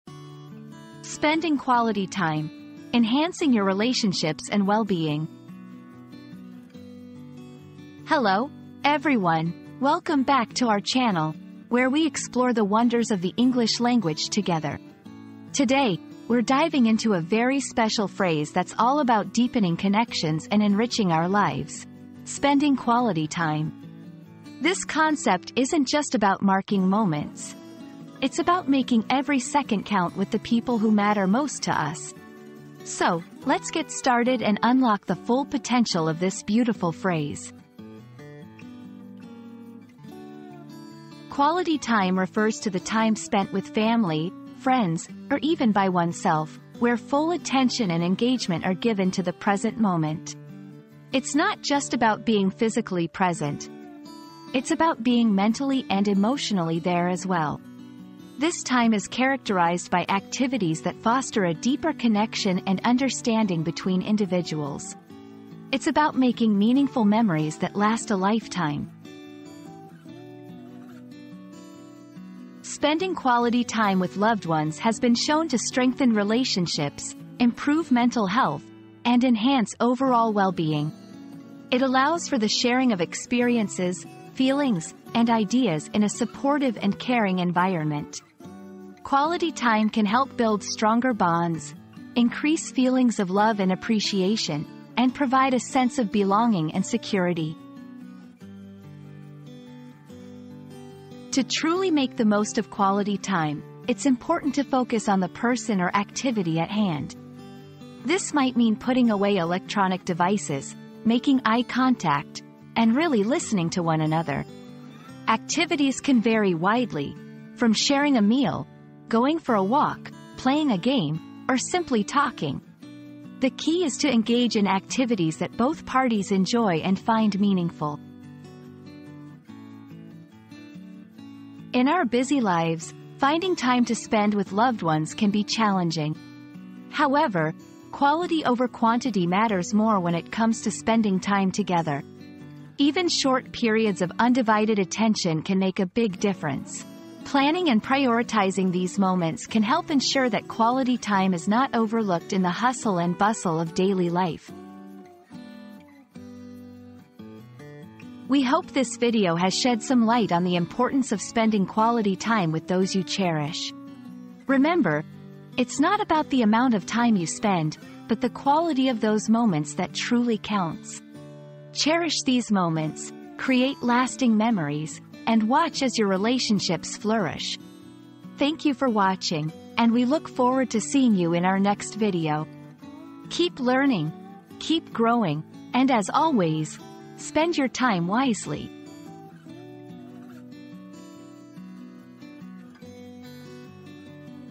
Spending Quality Time: Enhancing Your Relationships and Well-being
00:00 • Introduction - Spending Quality Time: Enhancing Your Relationships and Well-being
00:40 • Understanding "Quality Time"
01:12 • Why Quality Time Matters
01:40 • How to Spend Quality Time
02:11 • Overcoming Challenges